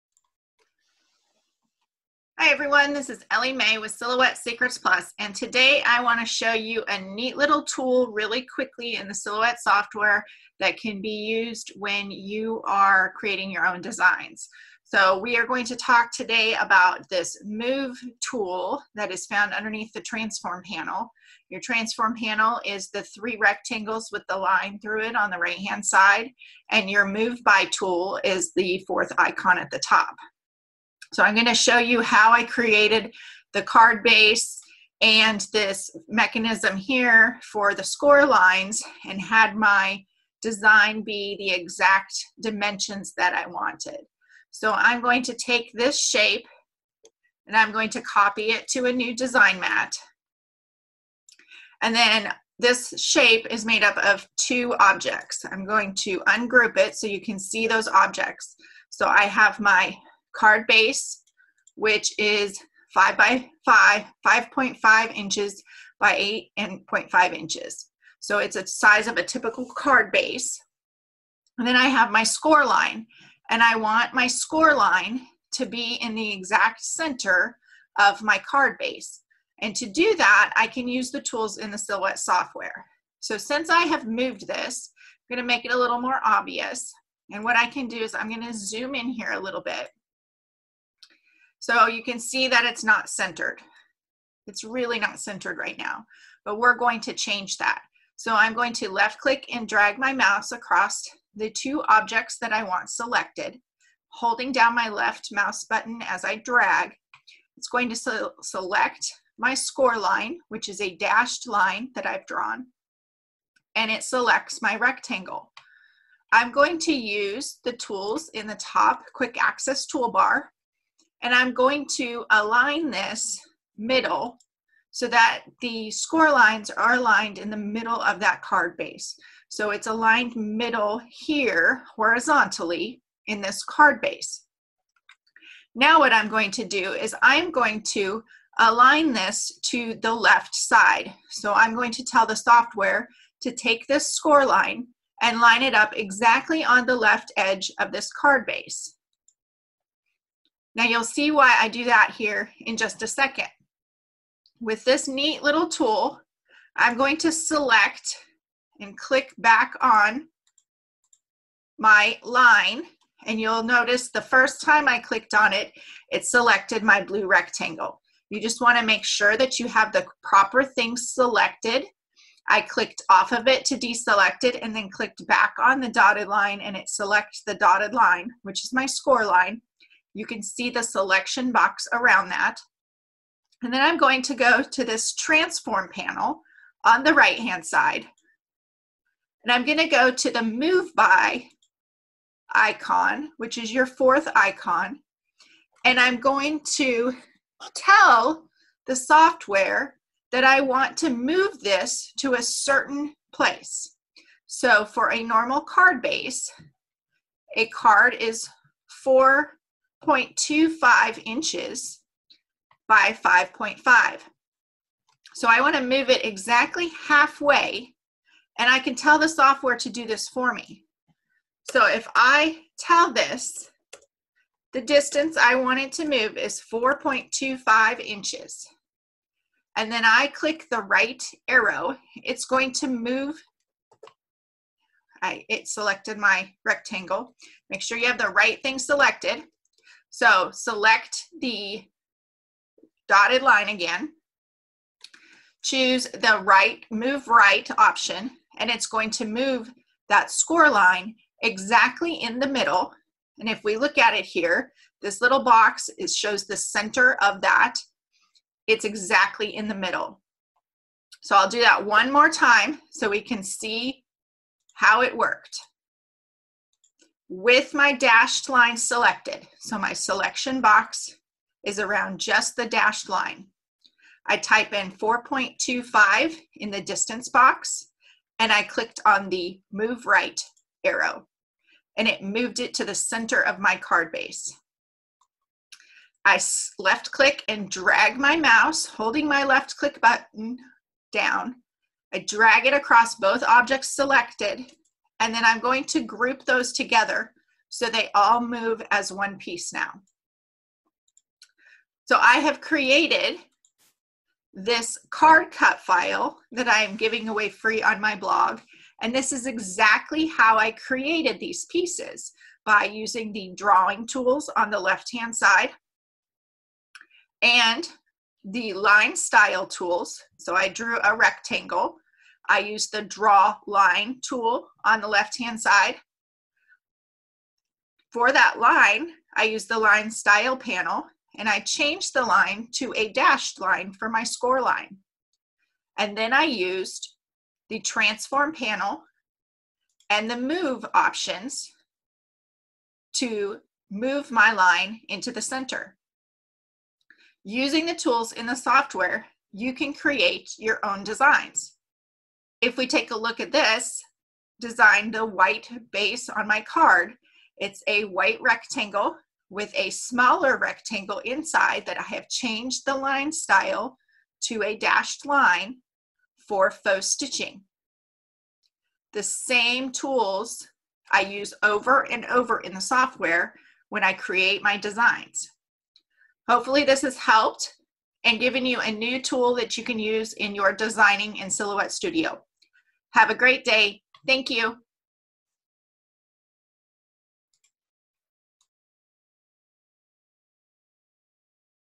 Silhouette Studio - Move By Tool
In the Silhouette software, there are many tools that can help you in designing. One of those small but powerful tools that I don't think is utilized often is the Move By Tool. Watch how it works here.
Find out more information and pick up this free card cut file on the blog